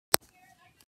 Dat 7 of the Christmas countdown
Join me as look a some old Rudolph action figure